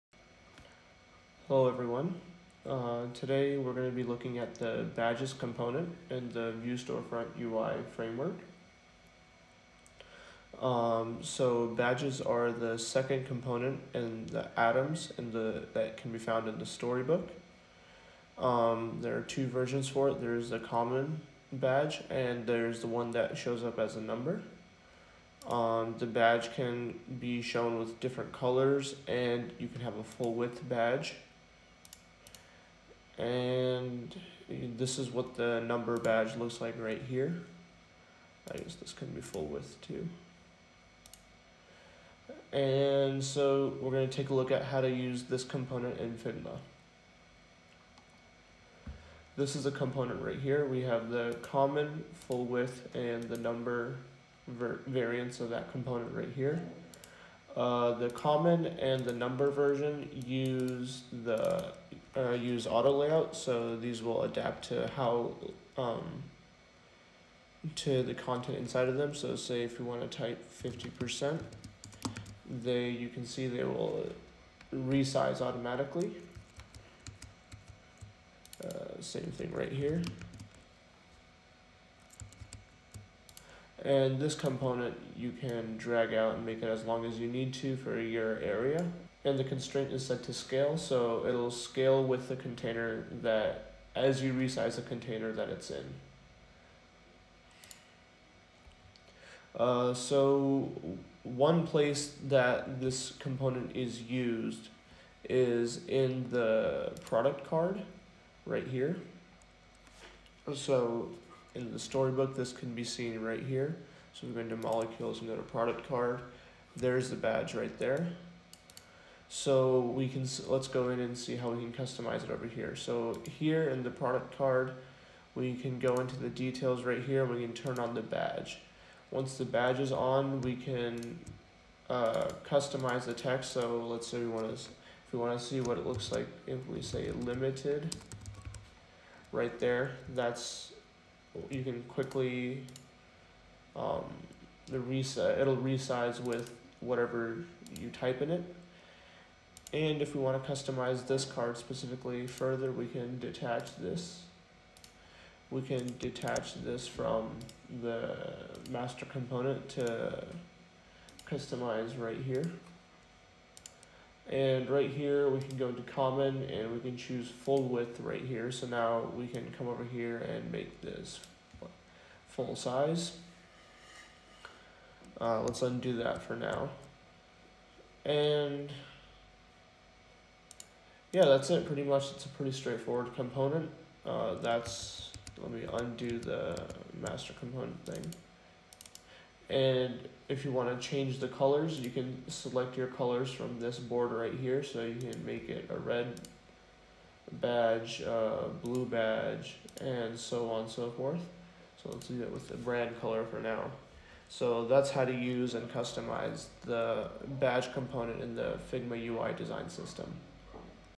Storefront UI Figma - Badge
This video covers the usage of the Badge component in the Storefront UI Figma file.